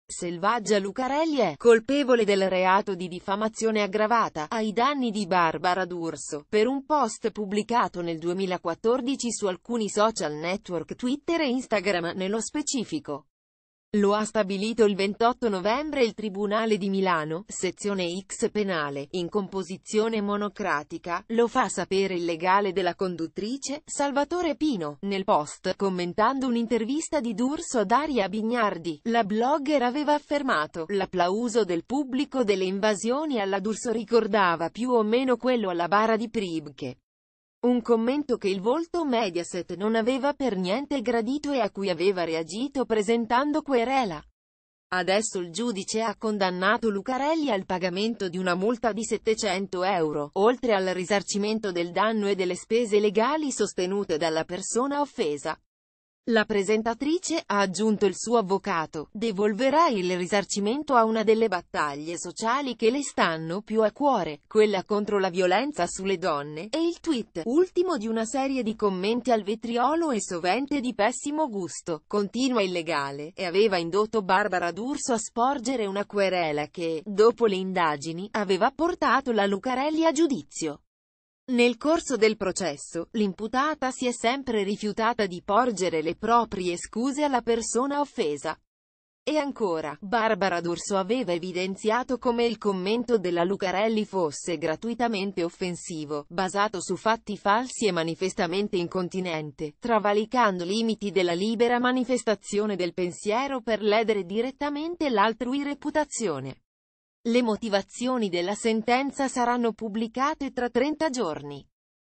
Savage Lucarelli ha diffamato Barbara di Urso sul sociale, la condanna della Corte di Milano
Selvaggia Lucarelli è «colpevole del reato di diffamazione aggravata» ai danni di Barbara D’Urso, pe...